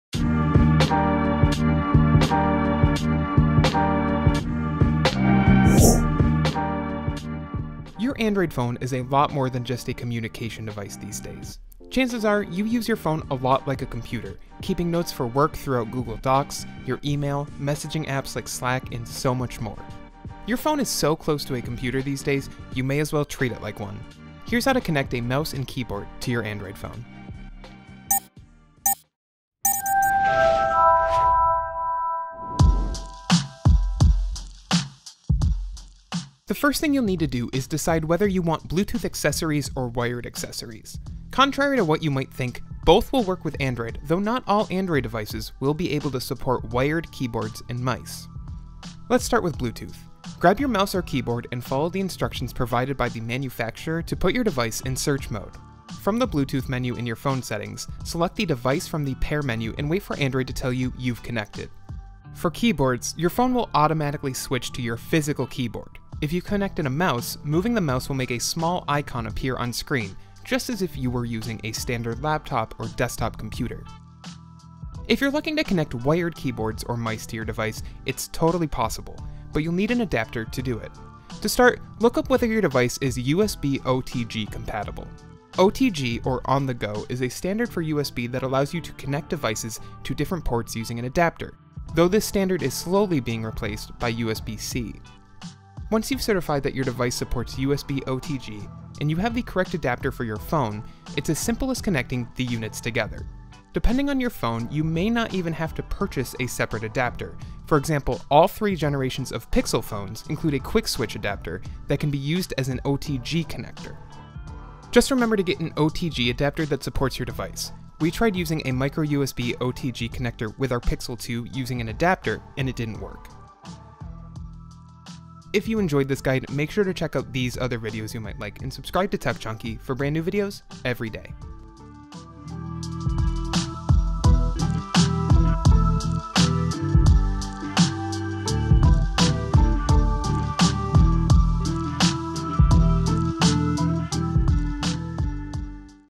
Using a Mouse and Keyboard with Your Android Phone!
Chances are, you use your phone like a computer, keeping notes for work throughout Google Docs, your email, messaging apps like Slack, and so much more. Your phone is so close to a computer these days, you may as well treat it like one. Here’s how to connect a mouse and keyboard to your Android phone.

Check out the full guide at TechJunkie.com!